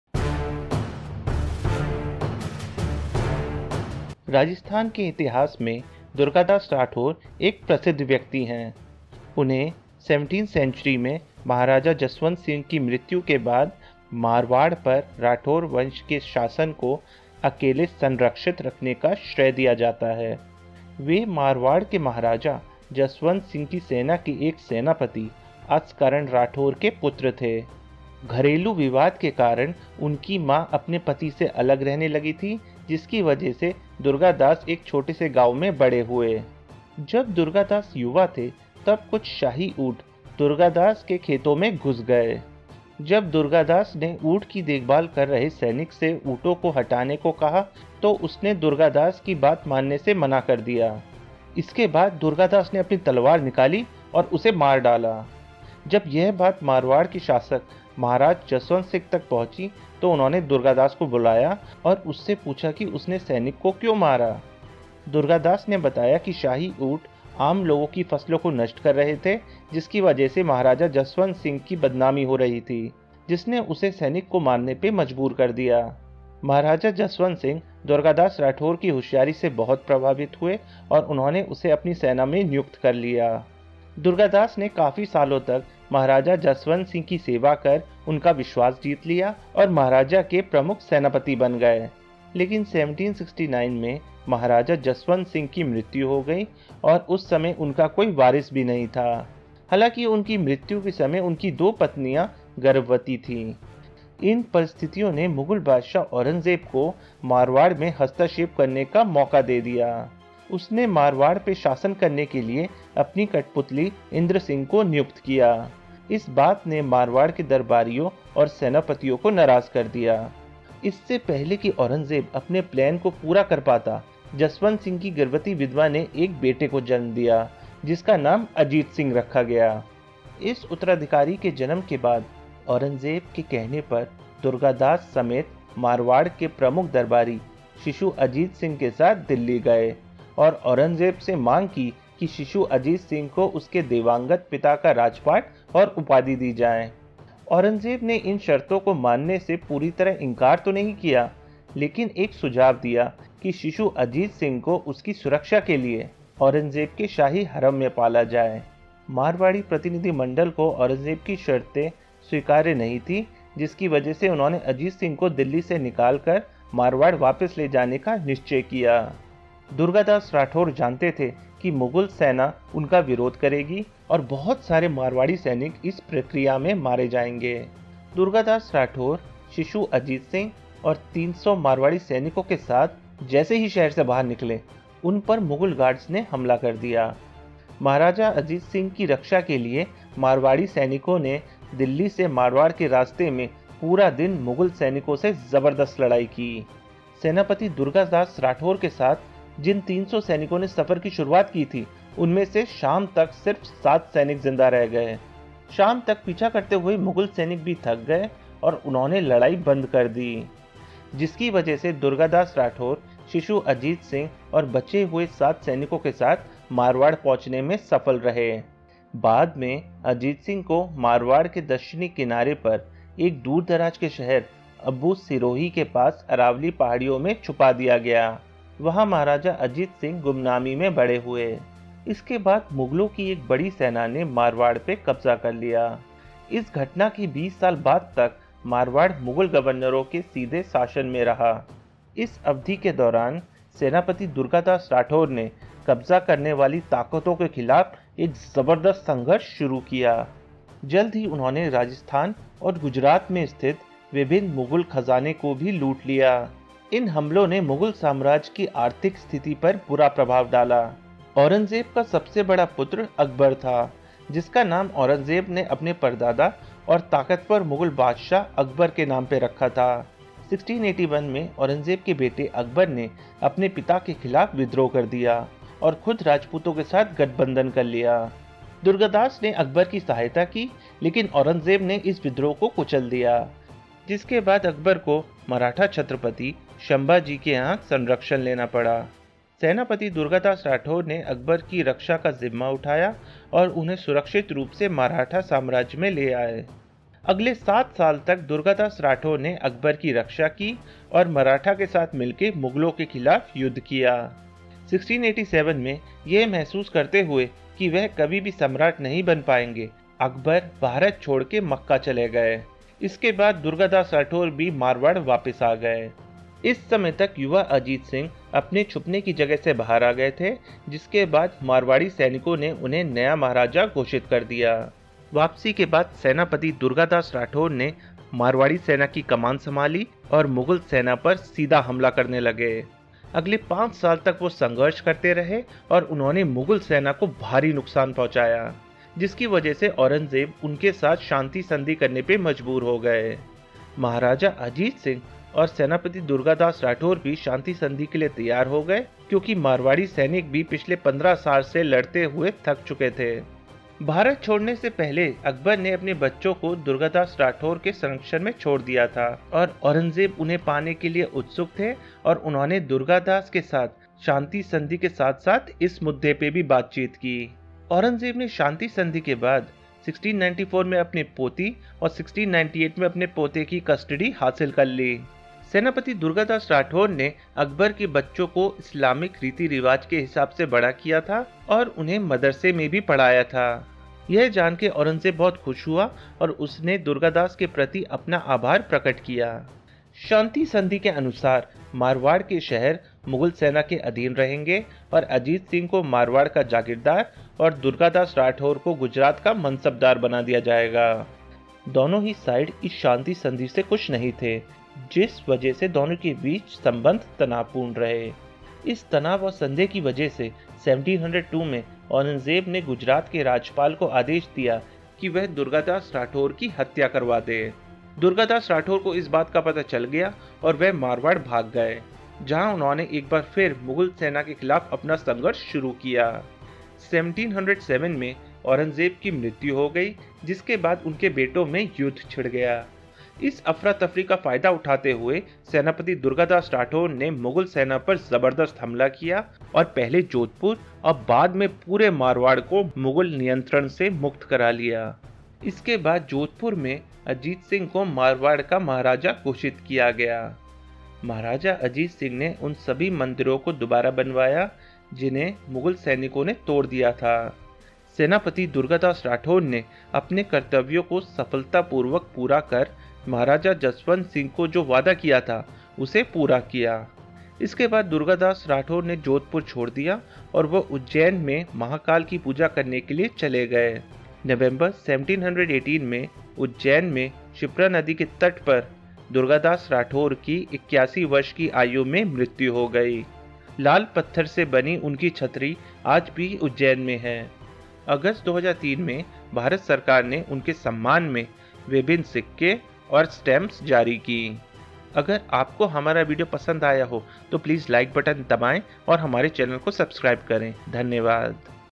Durgadas Rathore- मुगलो के खिलाफ मारवाड़ की सेना का नेतृत्व किया | durgadas rathore history in hindi
This video describe durgadas rathore history in hindi.

Durgadas Rathore is a famous personality in the annals of Marwar.
He is credited with having single-handedly preserved the rule of the Rathore dynasty over Marwar (Jodhpur) after the death of Maharaja Jaswant Singh in the 17th century.

He was the son of Askaran Rathore, a Rajput general in the army of Maharaja Jaswant Singh of Marwar.
As his mother did not get along with her husband and co-wives, and Durgadas grew up in a small village.